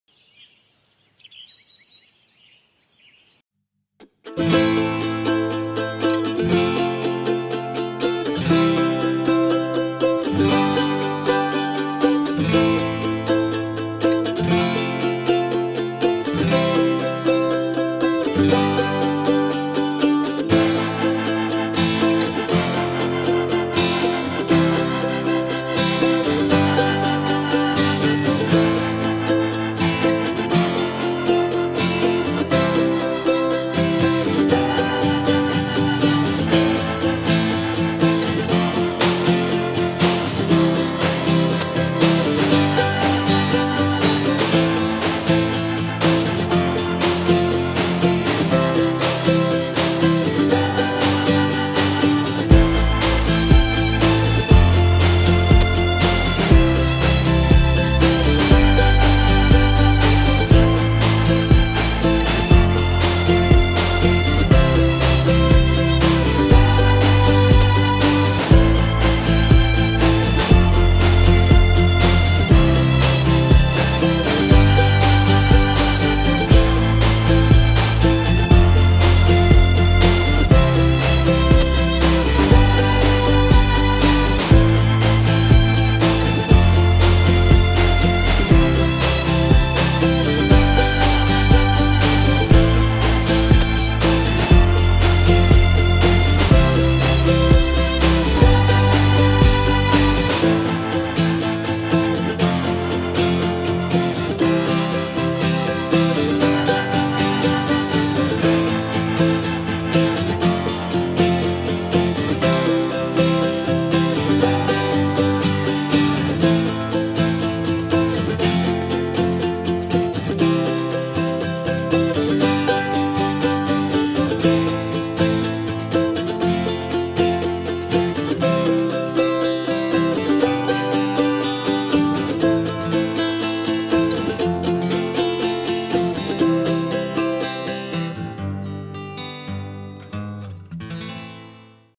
John Deere S670 e 9770 STS colhendo trigo
Colheita de trigo 2019, com as colheitadeiras S670 e 9770 STS.